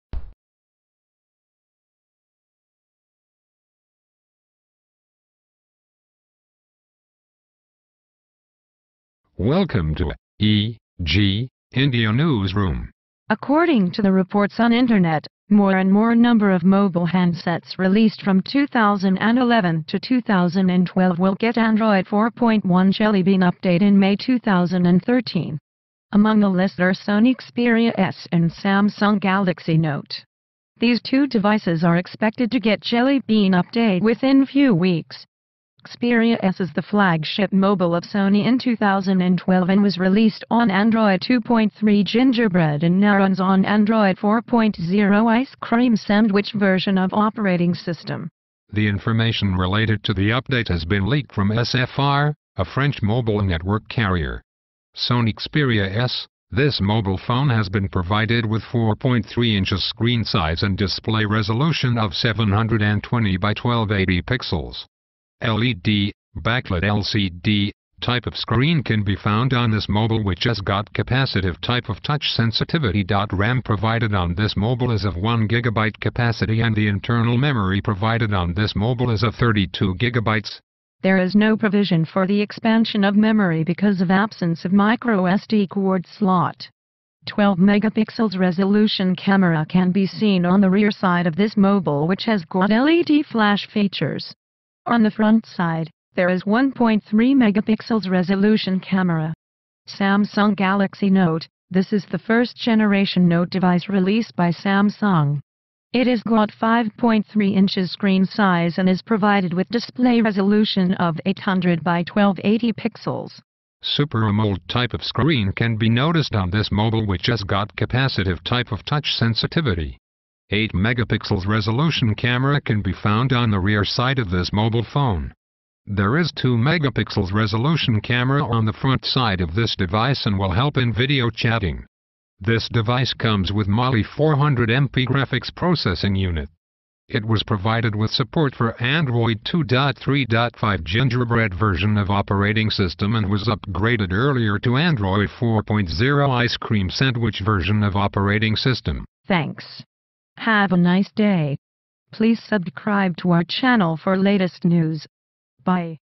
Sony Xperia S & Galaxy Note to get Jelly Bean Update Soon
Read Text Version According to the reports on internet, more and more number of mobile handsets released from 2011 to 2012 will get Android 4.1 Jelly Bean update in May 2013.